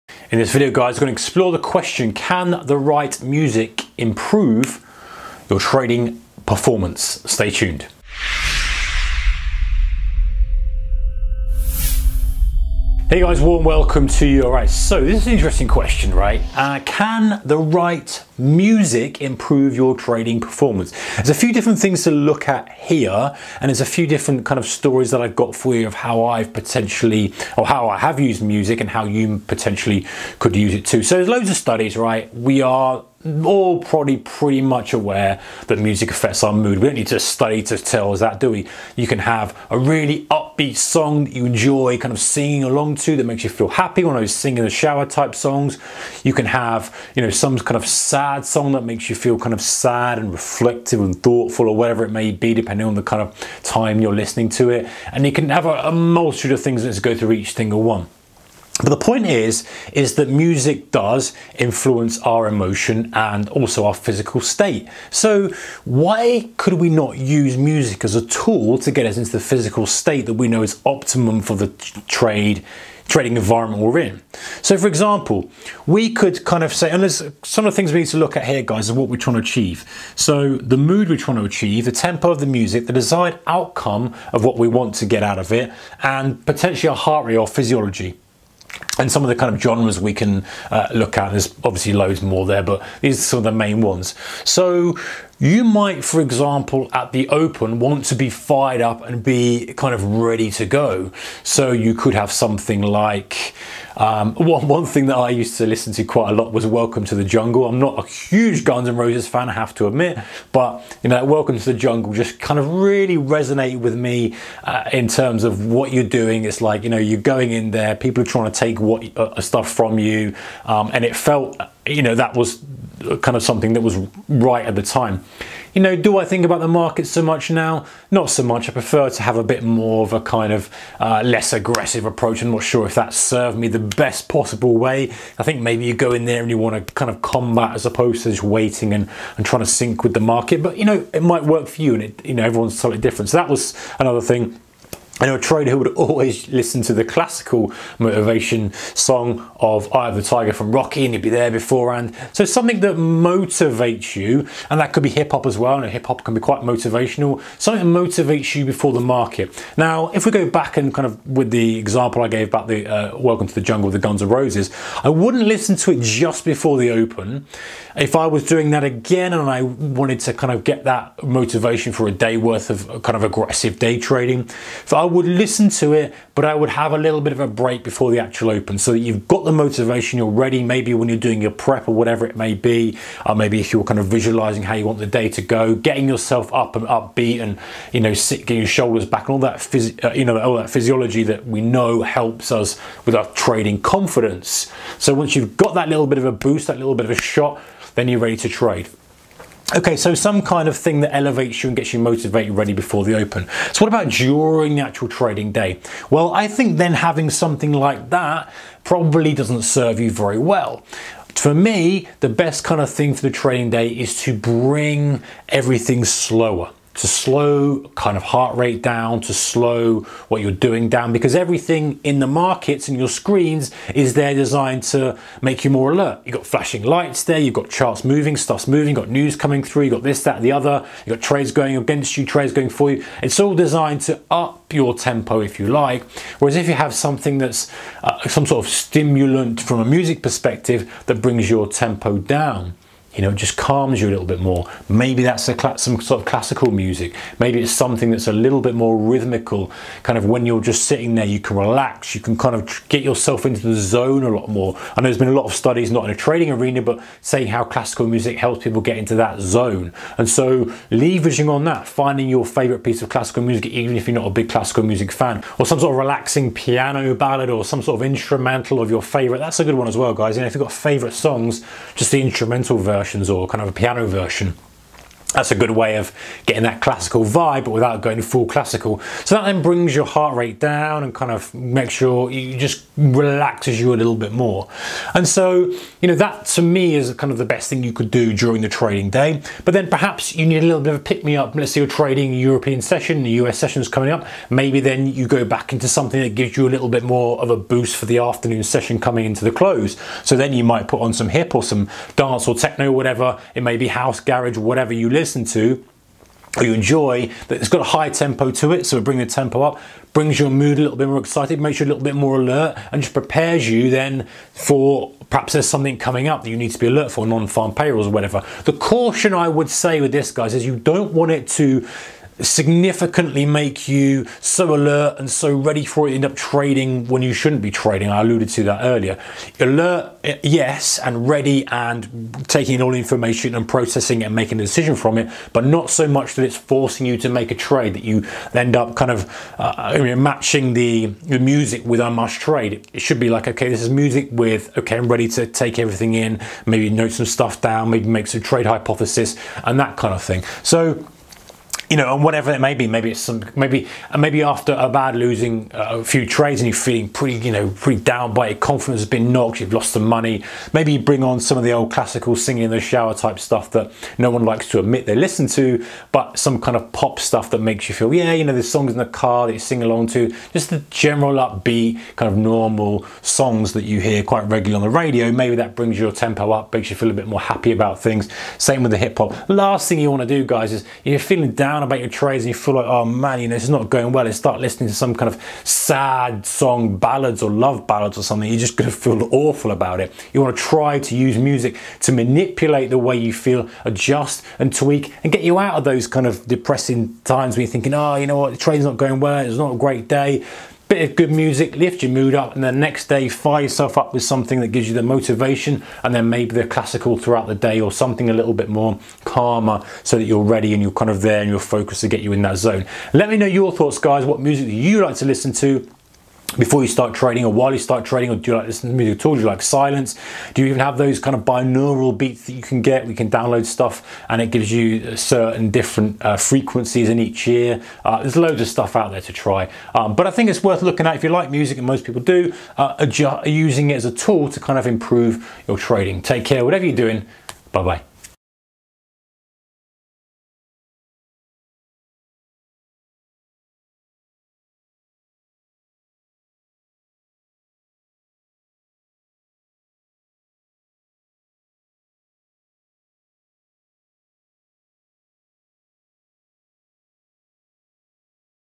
How Music May Improve Your Trading Performance! 🎸🎵
Could the right music improve your trading performance?  Can music improve your trading? This is interesting.  There are lots of studies done that demonstrate that music impacts our mood.  Some songs make us feel happy, sad songs help us reflect and think depending on the time you're listening to them.  The point is that music does influence our emotions and our physical state.  So why not use music as a tool to get into the physical state that we know is optimal for the trading environment we're in. OK.

Perhaps something that motivates you before the market open might be appropriate.  Having something like this during the course of the trading day might not serve you that well.  I try to bring everything slower then.

Mood
Tempo
Desired Outcome
Heart Rate

Classical
Rock
Hip Hop
Dance/Techno
Pop